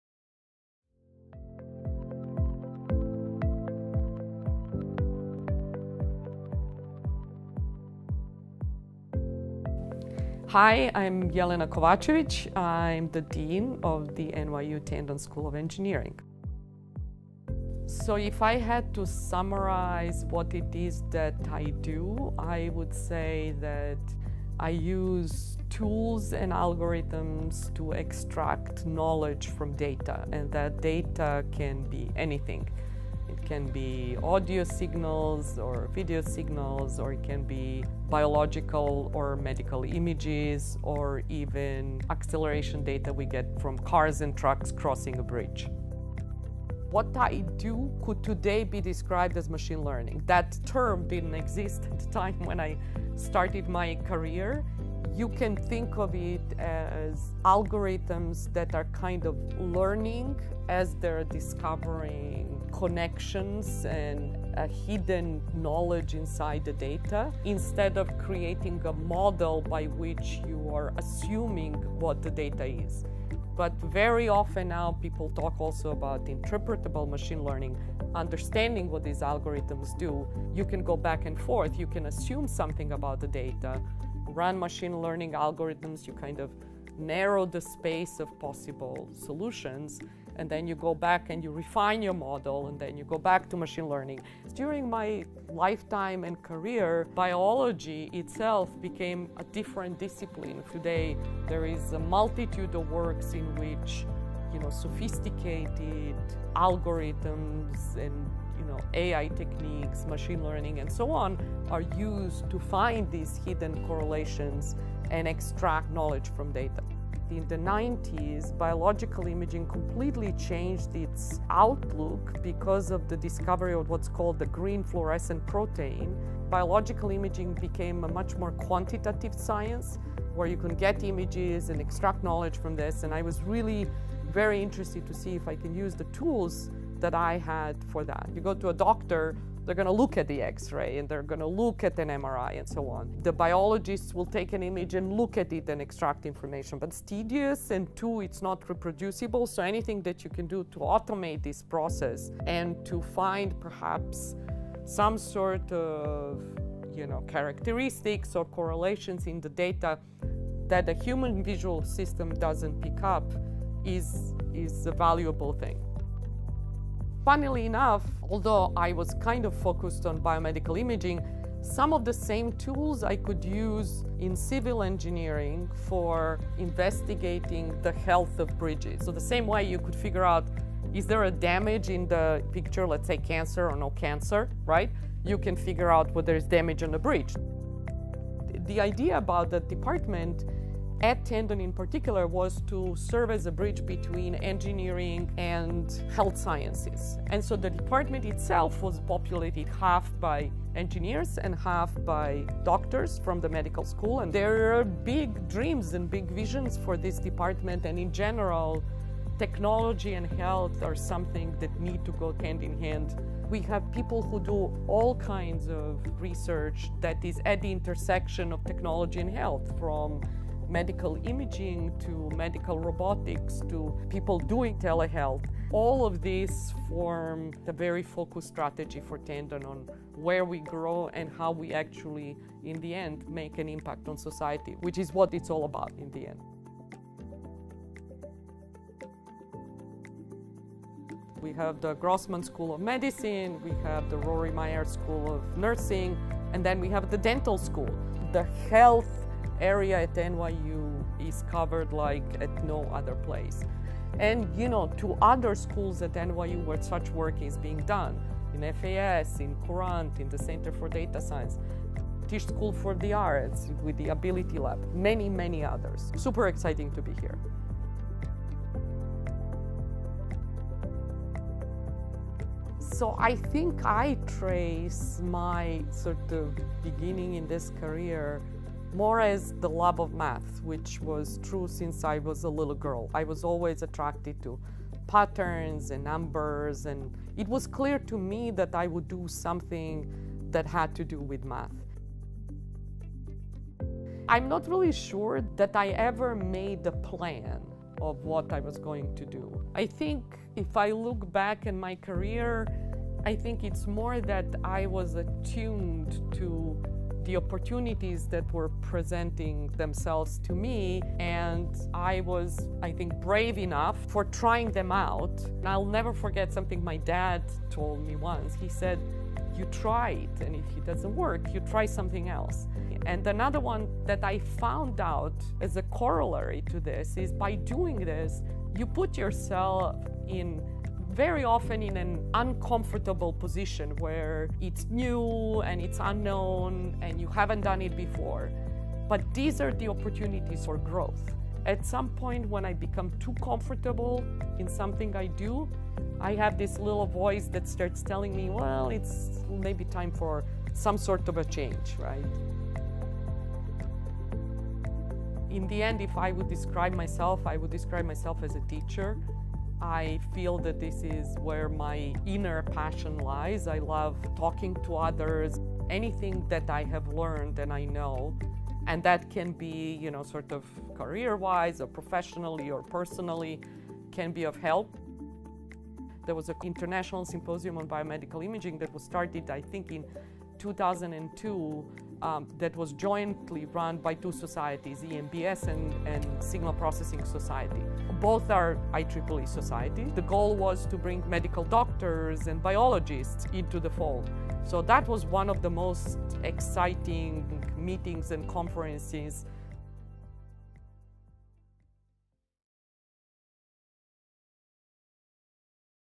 Career Reflections: Dean Jelena Kovačević
Dean Kovačević discusses the evolution of machine learning, her work to extract knowledge from data, the potential for biomedical engineering, and advice for up-and-coming engineers in honor of receiving a 2022 IEEE Engineering in Medicine & Biology Society Career Achievement Award for pioneering academic leadership in biomedical imaging research & education.